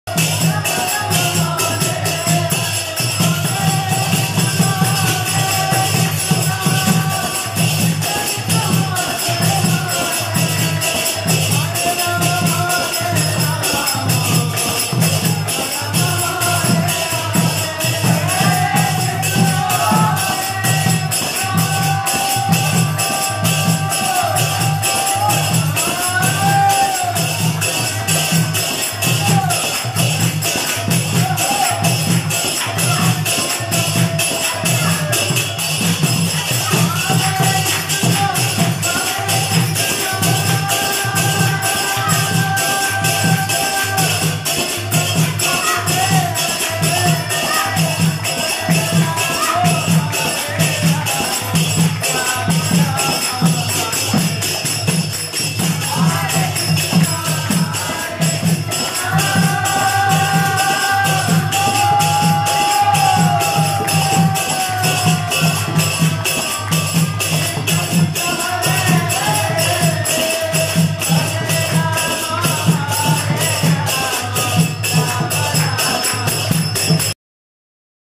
Ecstatic Kirtan by Brahmachari Devotees #1 at ISKCON BRAHMAPUR, October, 2014 | Hare Krishna Temple
Hare Krishna !
All glories to Sri Guru, Gauranga & Srila Prabhupada !!!
All glories to all the Devotees at ISKCON Brahmapur.